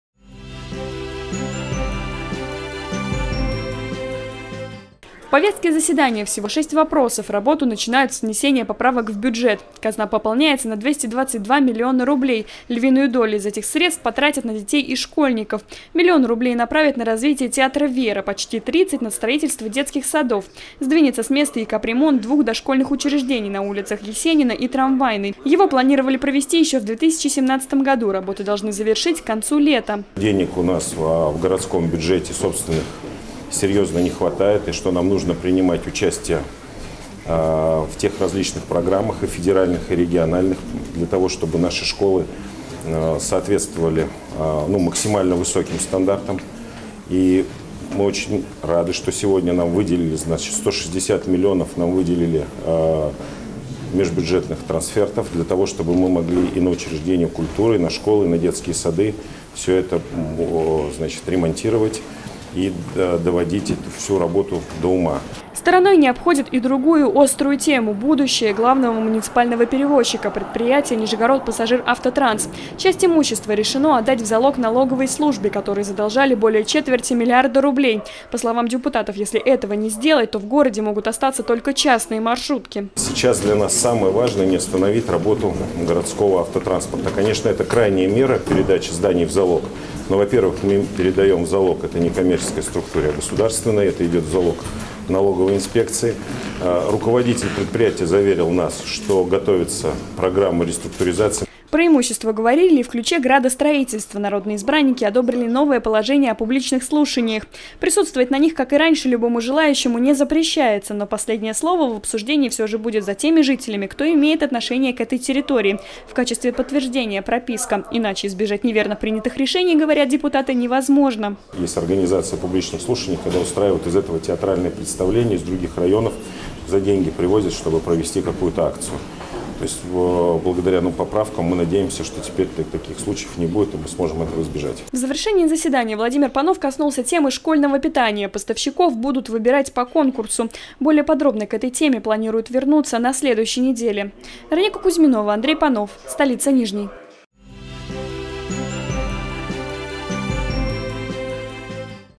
Депутаты гордумы одобрили поправки в бюджет Нижнего Новгорода на 222 млн рублей
11 июля 2018 года прошло внеочередное заседание городской Думы Нижнего Новгорода. На повестке у депутатов стояло всего шесть вопросов. Первый — о внесении поправок в бюджет города на 222 миллиона рублей. Часть из них направят на строительство детских садов, миллион из казны выделят на развитие театра «Вера». Народные избранники не обошли стороной и будущее предприятия «Нижегородпассажиравтотранс». Они одобрили передачу части имущества НПАТ в залог налоговой службе. Подробности — в материале «Столицы Нижний».